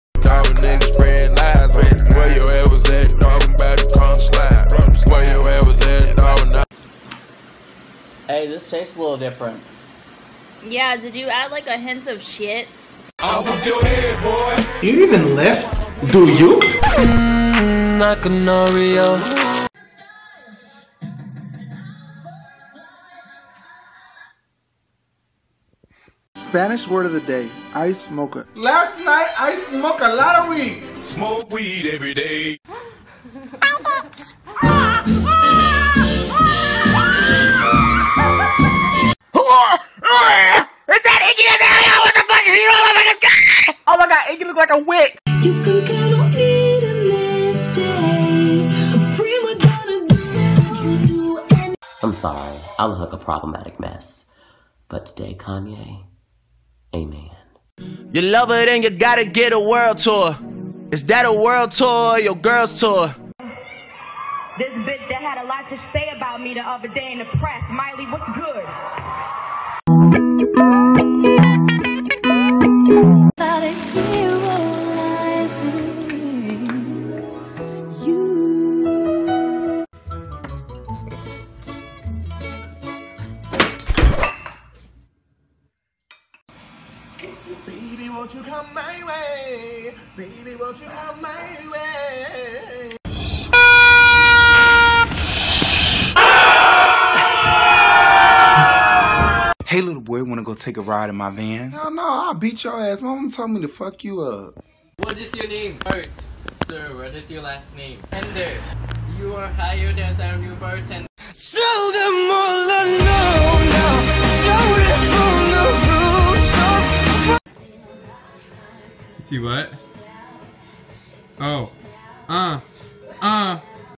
Best Vines of Hour #12 | 31 August 2015 | Top 20 Vine Compilation by Best Viners /w Titles
Best Vines of Hour 31 August 2015 #12 Top 20 Vines Compilation

Titles:
1 - Up to date street performer
2 - Do You Lift  Throwback
3 - I WISH I HAD AN EXPLANATION FOR THIS
4 - HAHAHAHAHAHAHA LMFAO LOOK AT MILEY THO
5 - When you dont want to eat food that looks gross... Lele Pons, JoJoe
6 - The magical Aaron Doh in his natural habitat
7 - This is when I turn off the VMAs smh
8 - Spanish word of the day Ice Mocha.  Vine By Jay Mendoza
9 - IM CRYINGG Tina woodsss edits
10 - BOP
11 - Why meek look like a supportive ass girlfriend
12 - When I saw Kanye. RIP TO ME ILOVE HIMMM vmas2015
13 - Happiness
14 - When you are applying for a job as a bartender and your name is Bart Ender. comedy bartender LOL relatable haha
15 - That one friend that goes too hard at parties
16 - IGGY CAME FROM THE PITS OF HELL YALL WHAT THE FUCK IS THAT
17 - Kids nowadays be like...
18 - Siblings  how to tell someone nicely their cooking sucks
19 -
20 - The Hero